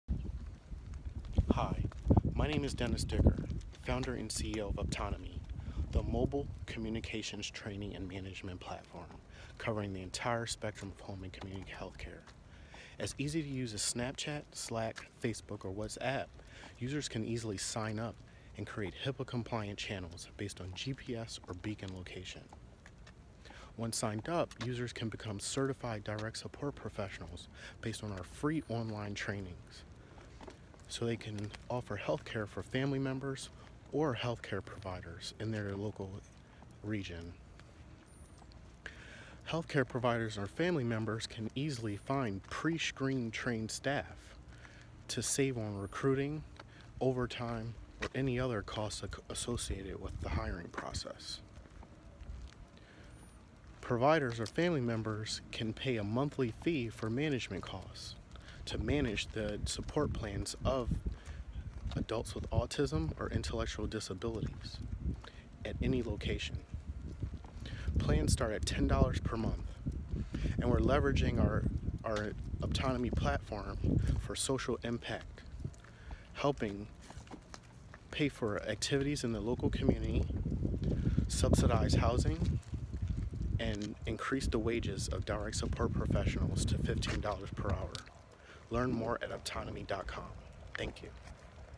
Optonome Introduction
The Only Mobile, Communications, Training and Management Platform Covering The Entire Spectrum Of Home and Community Healthcare